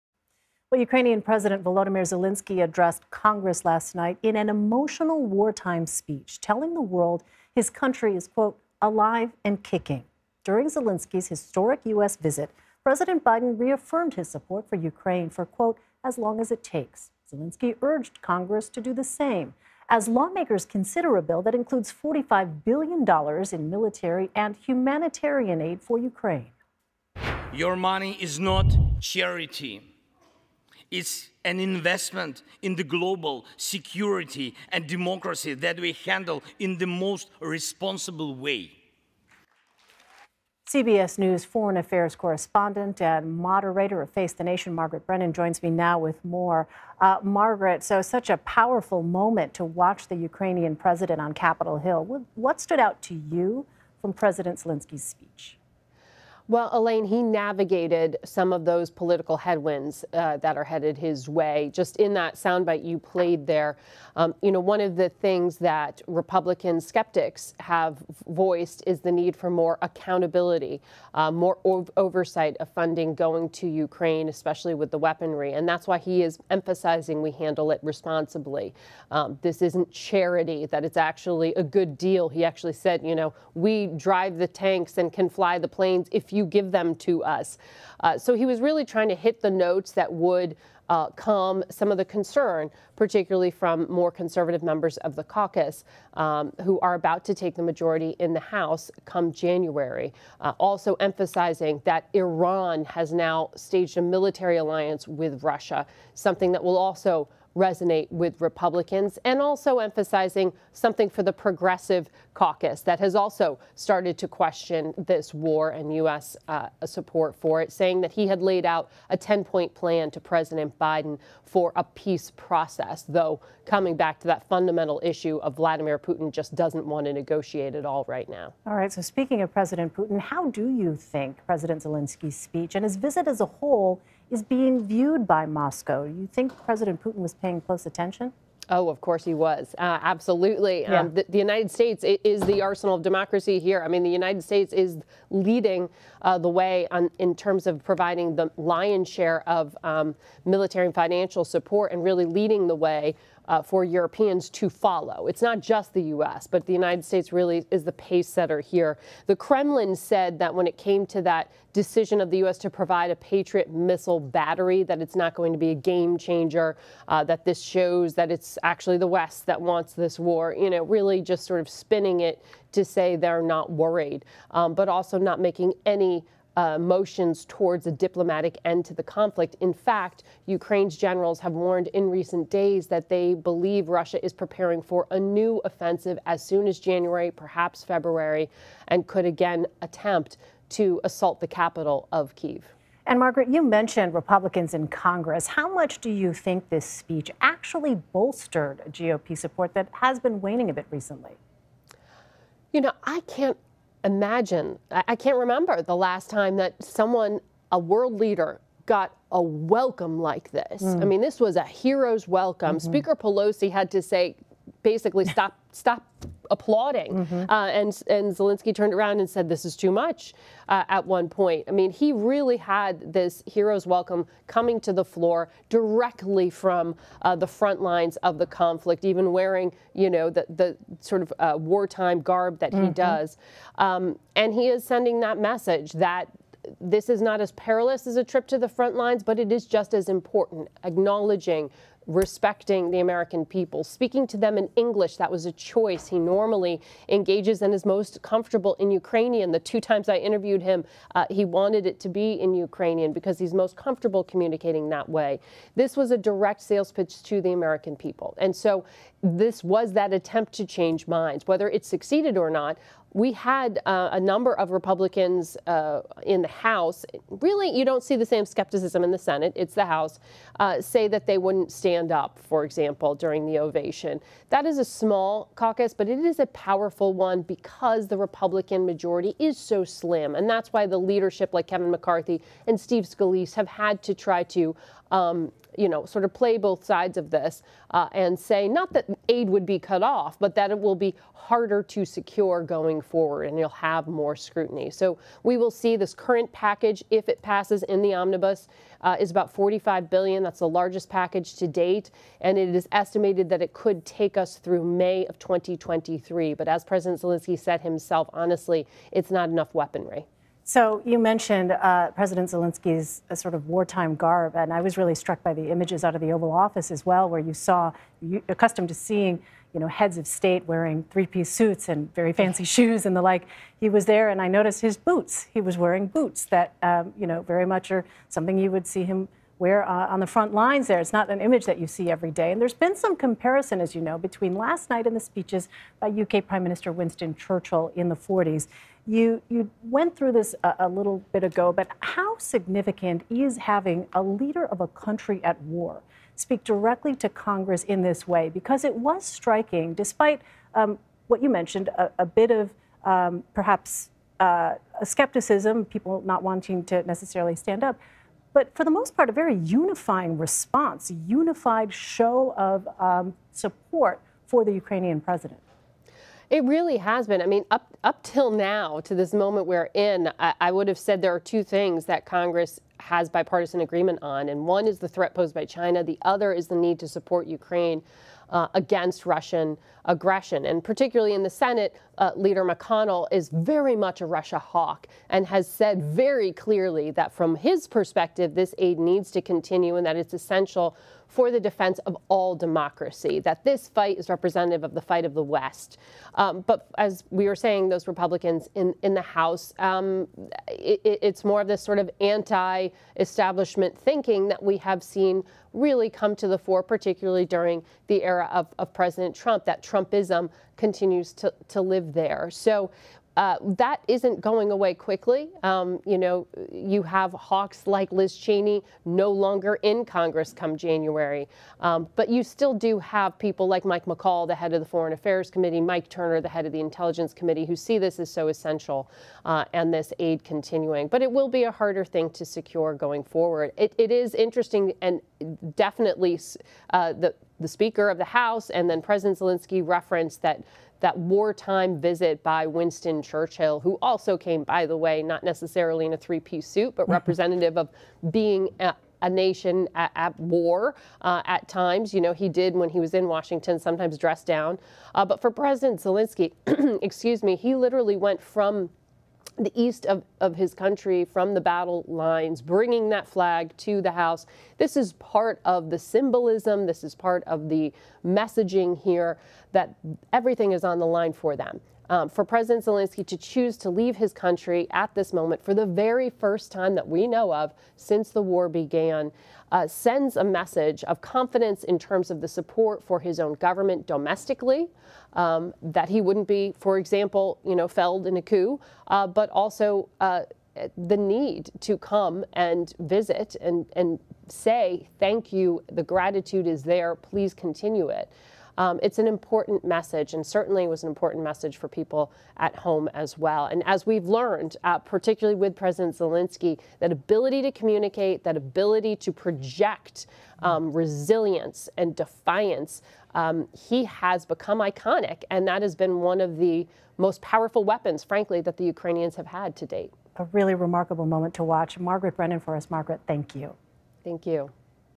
Ukrainian President Zelenskyy seeks to inspire and reassure Congress with wartime speech
Ukrainian President Volodymyr Zelenskyy thanked the U.S. for its support, calling aid to his country an investment in democracy. CBS News "Face the Nation" moderator and chief foreign affairs correspondent Margaret Brennan talks about how the Kremlin is likely taking Zelenskyy's visit, and how much his address to Congress may help bolster U.S. support.